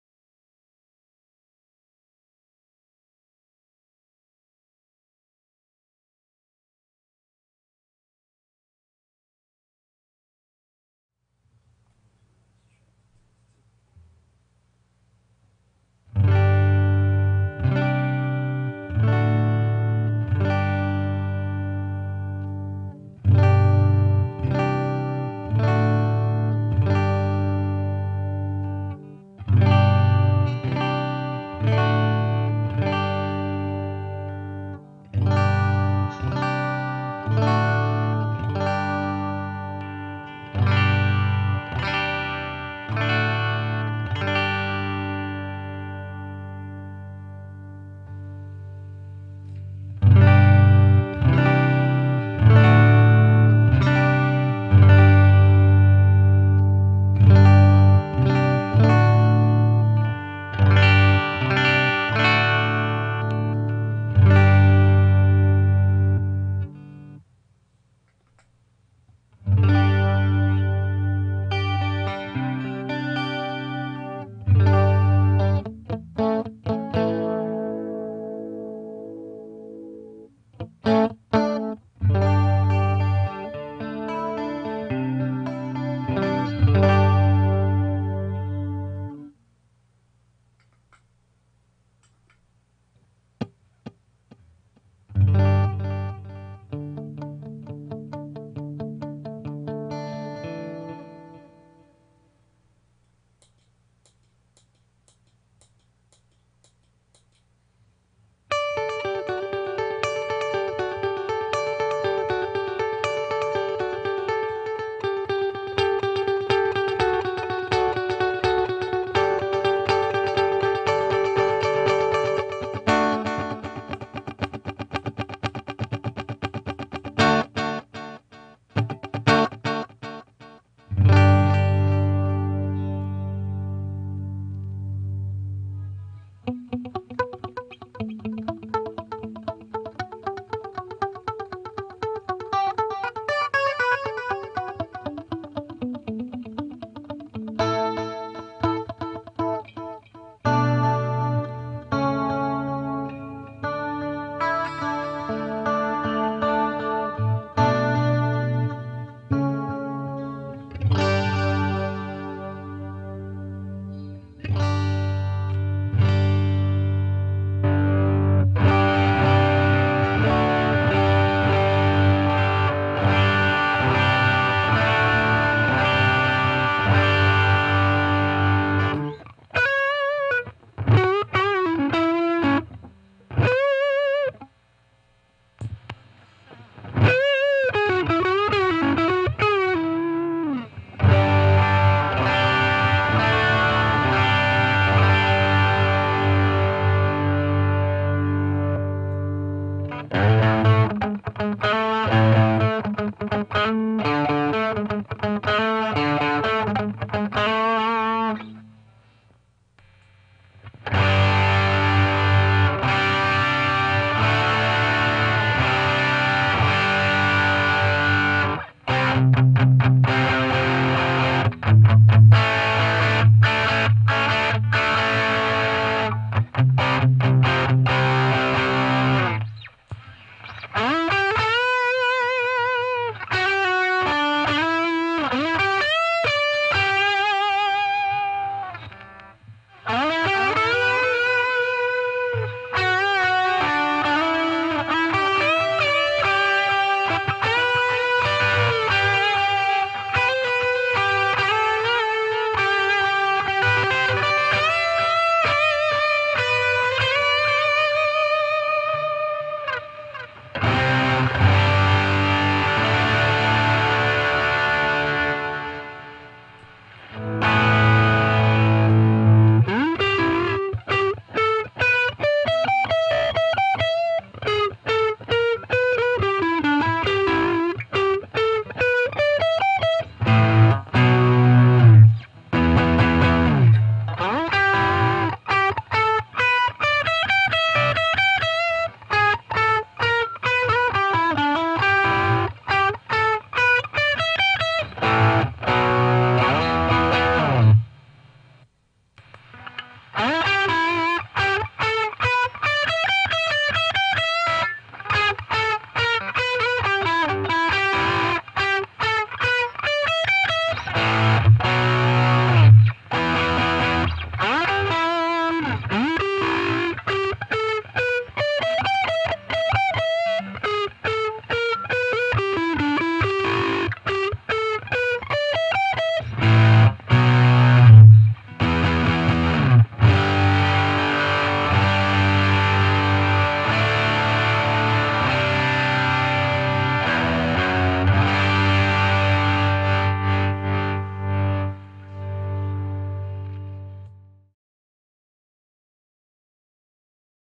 My Other Pedalboard Demo 2009
my smaller setup

DOD milkbox compressor, ibanez TS9, proco rat 2, boss CE-2, boss DD-5

better audio, still not good playing.